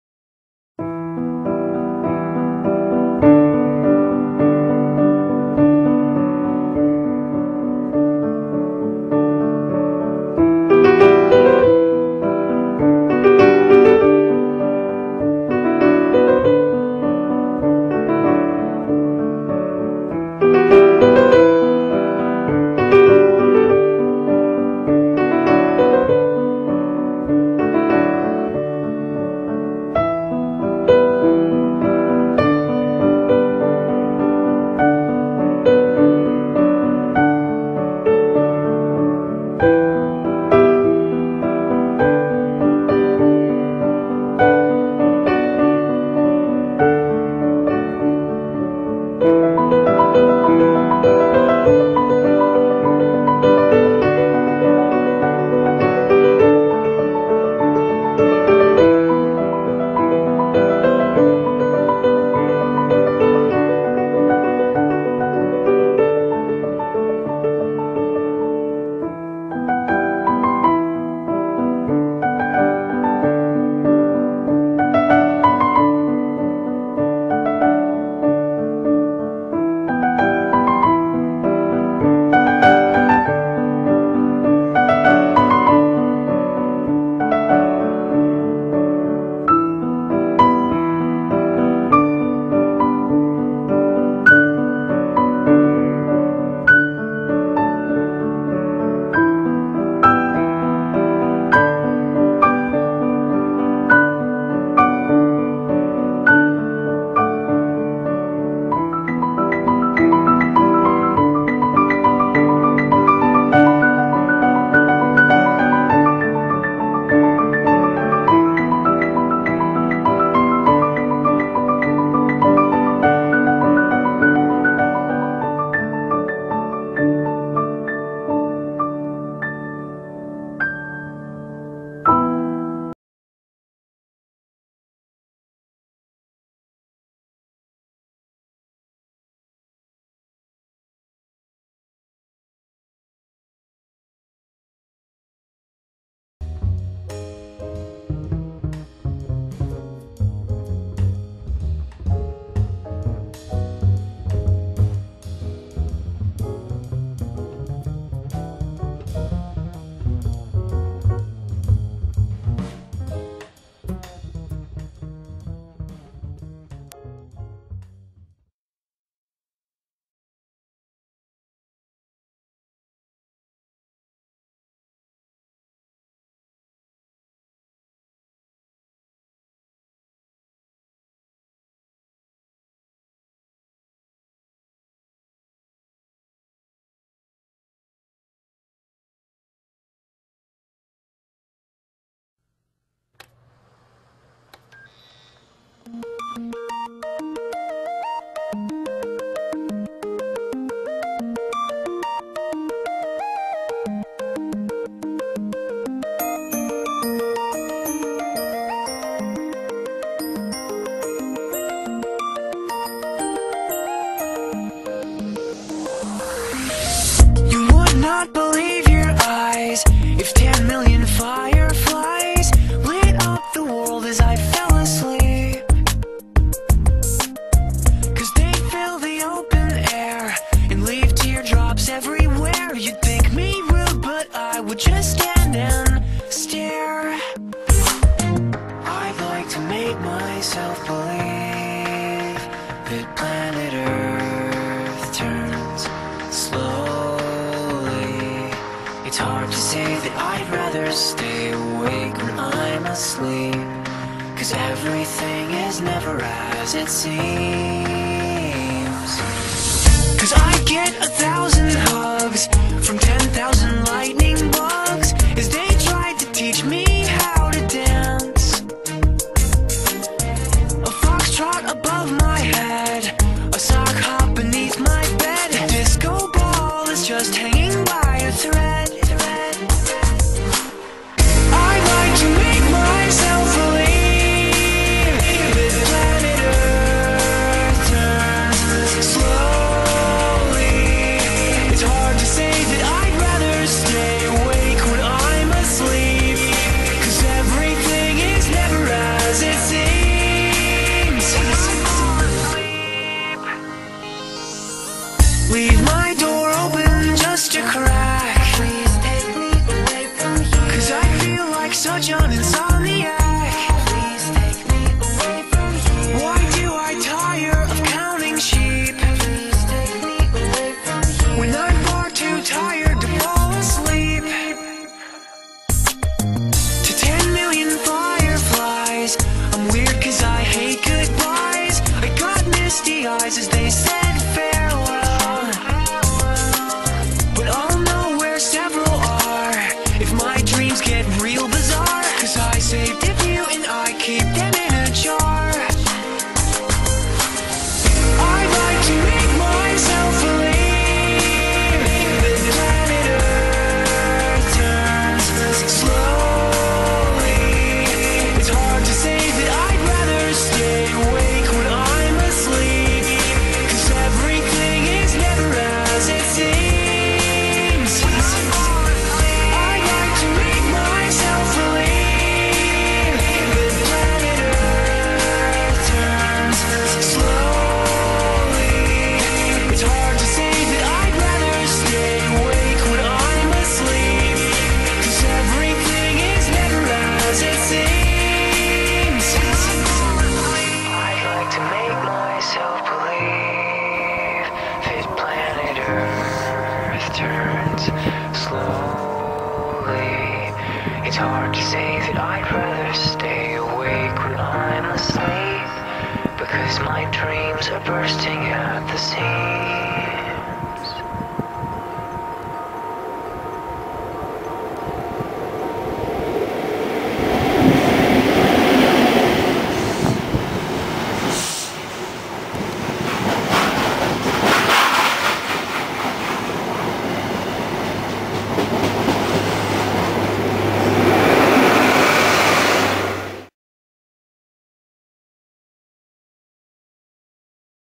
HST'S WILL NEVER BE FOR GOTTEN
H S T liverys
GNER
GRAND CENTRAL
Cross Country
EAST COAST
East Midlands

class 43's over the years in differnt liveries
not all are my pictures